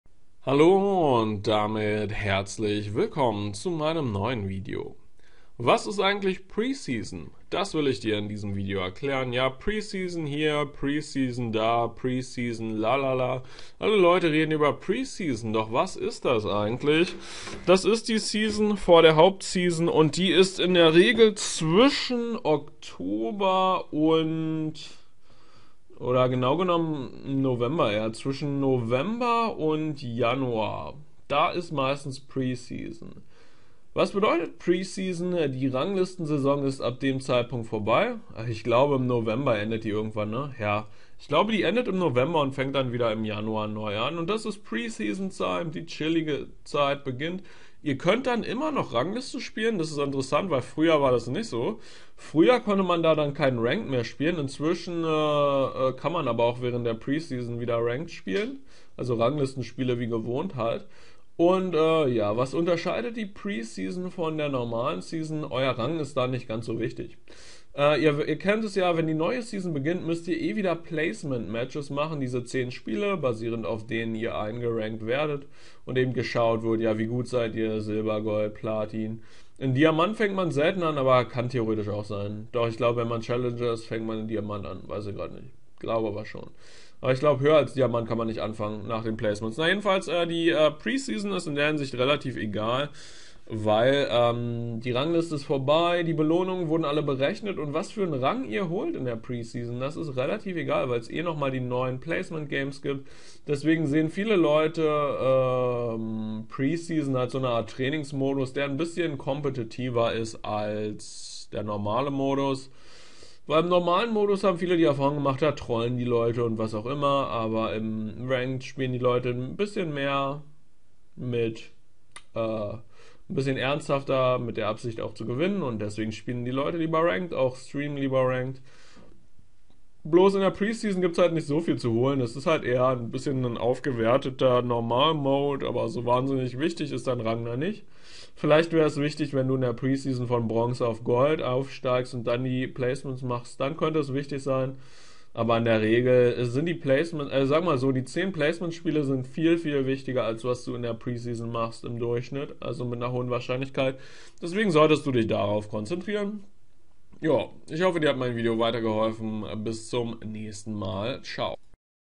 🍀 League of Legends: was ist Preseason? Von wann bis wann ist Preseason? Wie lange? 🍀
▶ Gut zum Zocken